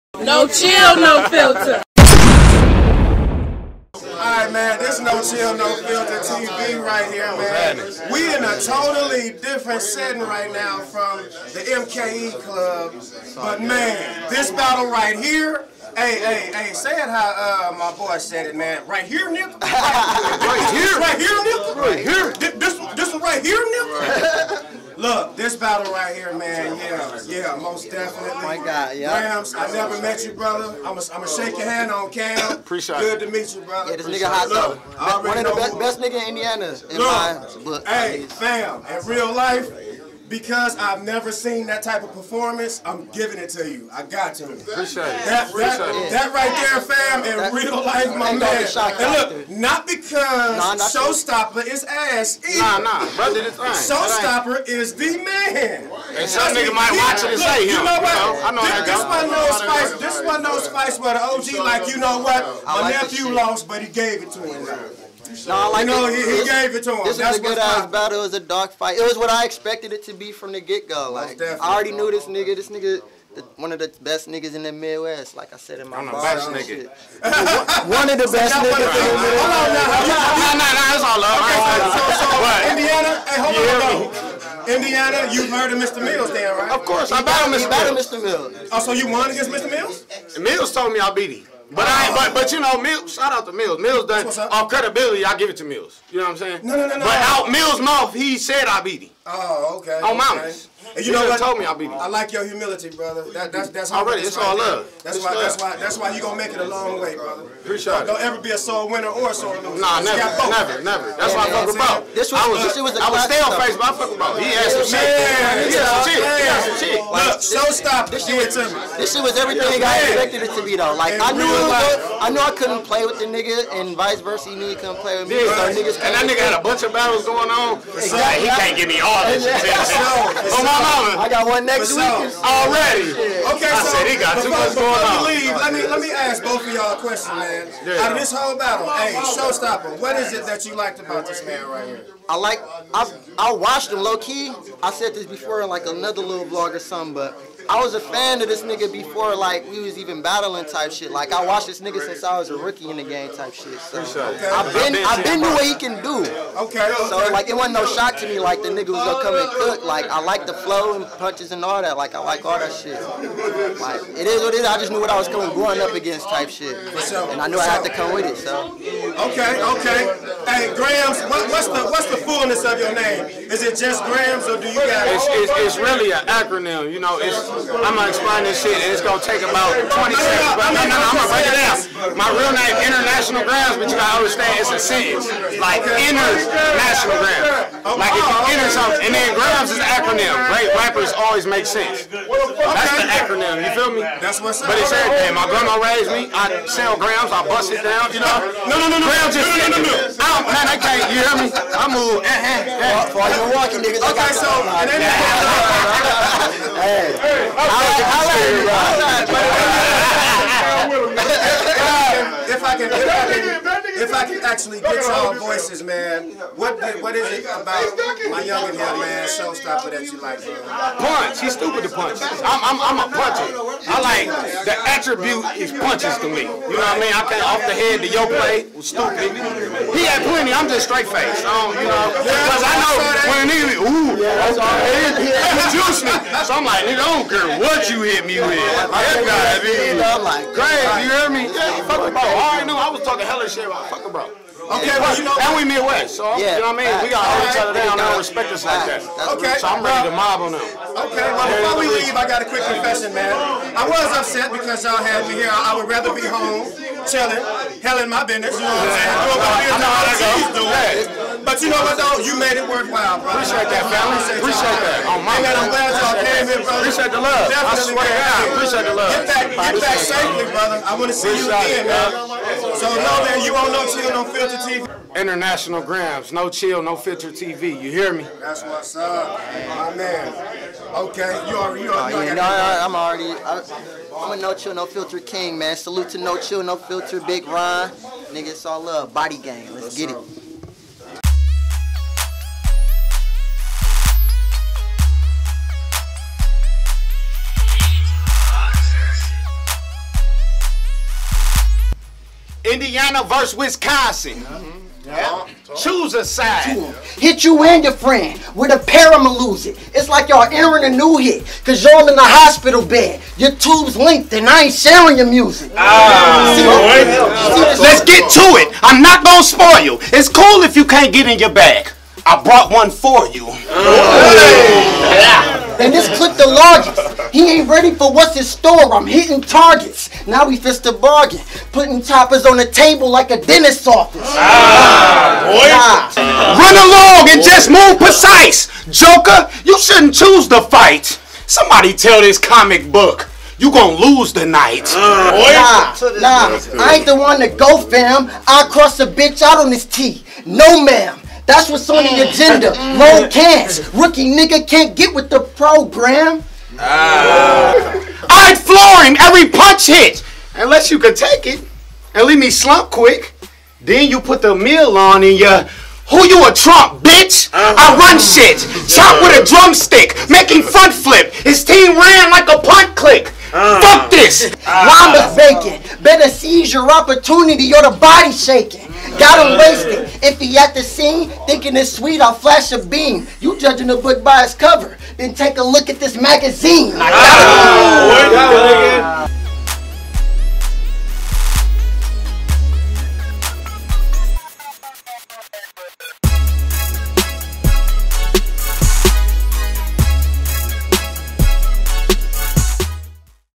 noChill noFilterTV | Heatrock battle event with Show Stoppa and Grams aftermath respect vlog
Show Stoppa and Grams chop it up in this noChill noFilterTV exclusive after the very explosive battle they were engaged in with teaser footage at the end with all first round schemes bars and haymakers courtesy of noChill noFilterTV